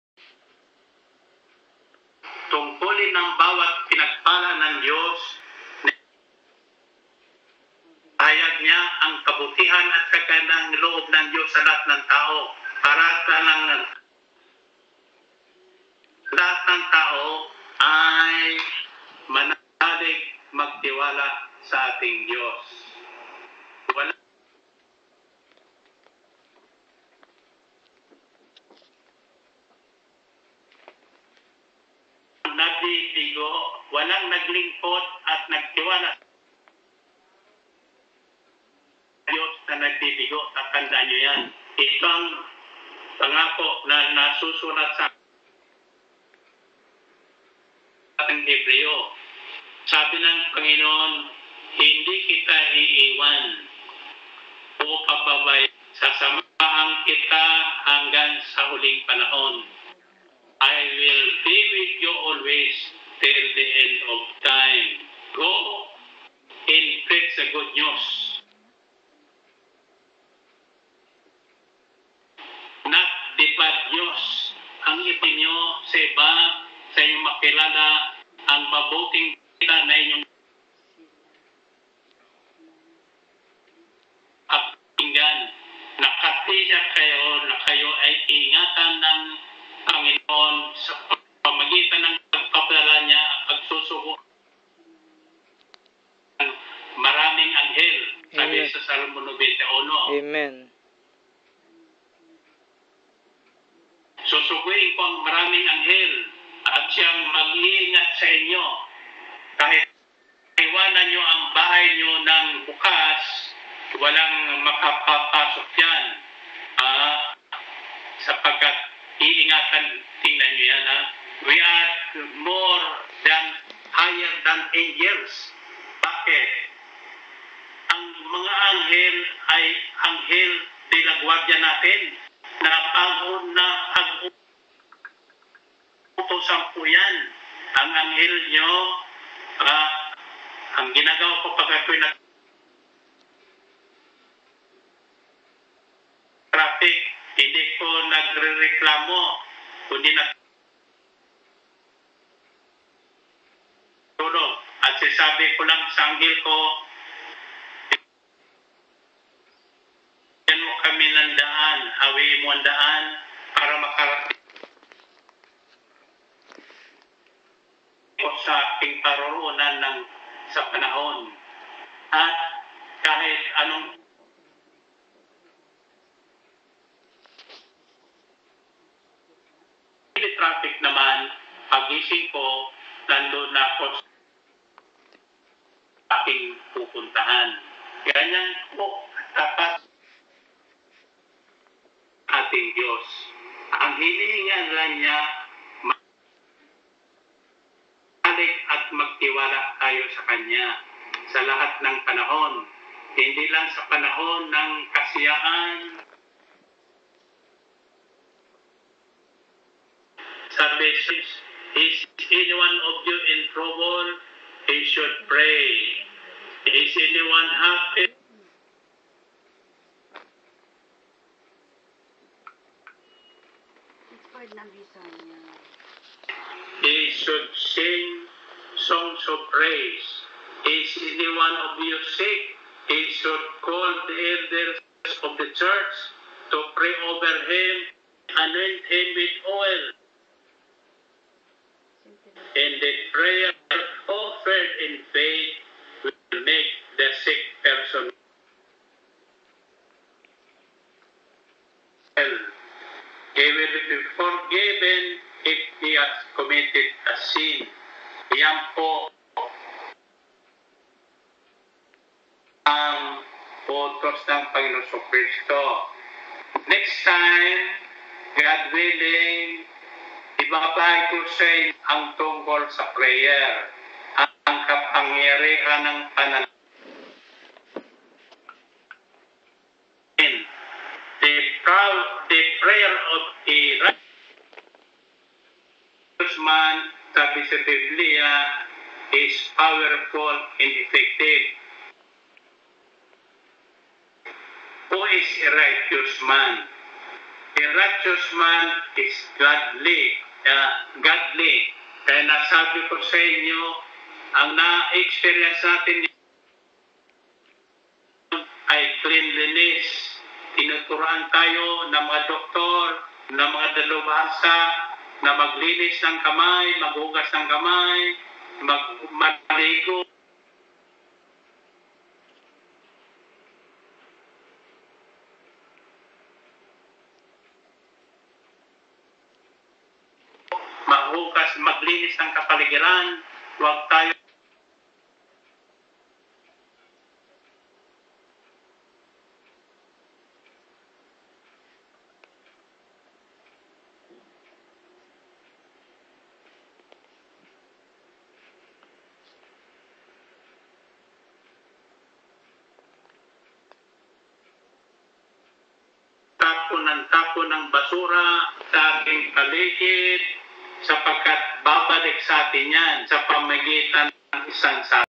PROCLAIM THE GOODNEWS OF SALVATION BY BRO MIKE VELARDE / GO AND MAKE DISCIPLES OF ALL NATIONS-JESUS
AS A BELIEVER OF CHRIST WE MUST PROCLAIM THE GOODNEWS OF SALVATION BY BRO MIKE VELARDE / GO AND MAKE DISCIPLES OF ALL NATIONS-JESUS / APRIL 4, 2020 VIA FB LIVE CREDIT TO THE OWNER